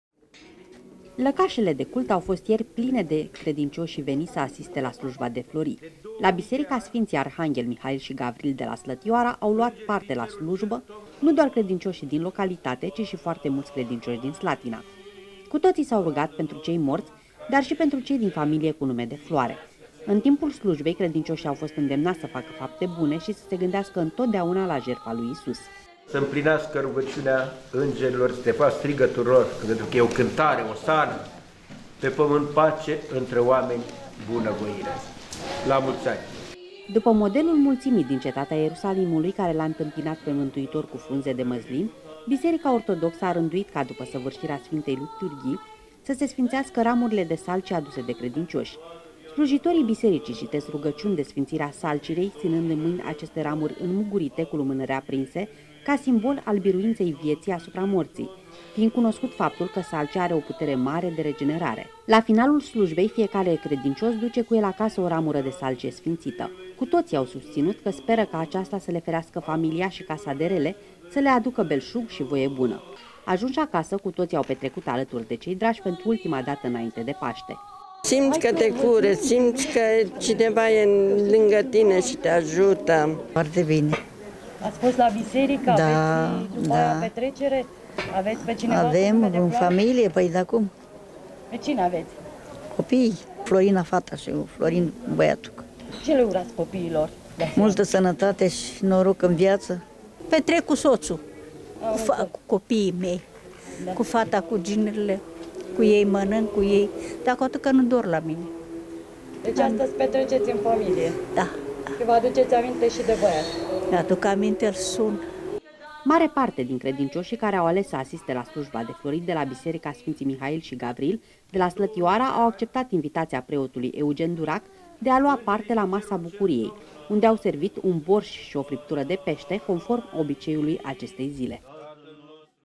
Credincioșii ortodocsi au sărbătorit Floriile
Creștinii ortodocși au celebrat, ieri, Floriile, cea mai importantă sărbătoare care vesteşte Paştele, evocând intrarea lui Iisus în Ierusalim, dar și una a celor cu nume de floare. Lăcașele de cult au fost pline de credincioși, unii rugându-se pentru cei morți, alții pentru membrii familiilor cu nume de floare. După slujbă, credincioșii ortodocși au plecat spre casele lor cu ramuri de salcie și au început petrecerea.